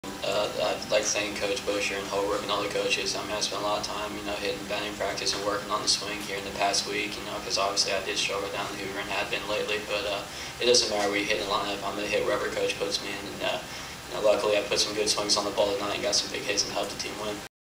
Saiko order
USC outfielder Graham Saiko gives his reaction to being moved lower in the batting order for Friday's game against Saint Louis.